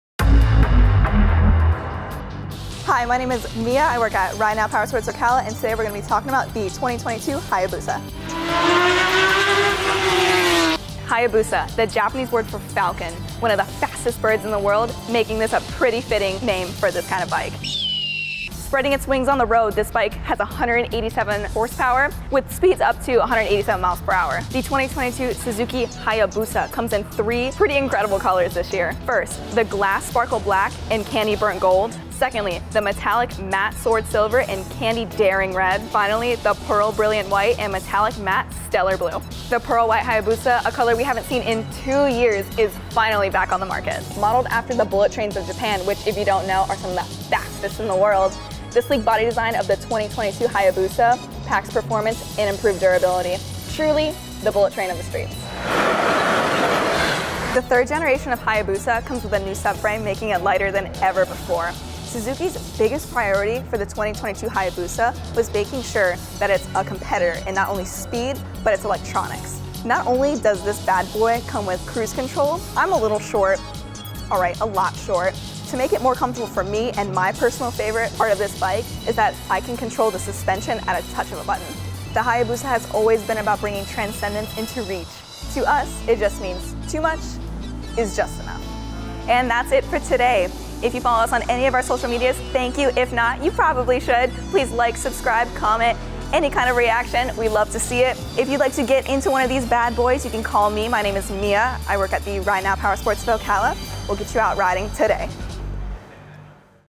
2022 Suzuki Hayabusa BREAKDOWN
The 2022 Suzuki Hayabusa comes with (3) bold color choices, a new twin-spar aluminum frame and aerodynamic redesign!

Need more info? I’ll hand this over to Mia from our WebSales Team to fill you in- take it away, Mia!

Our WebSales Team would be happy to assist you!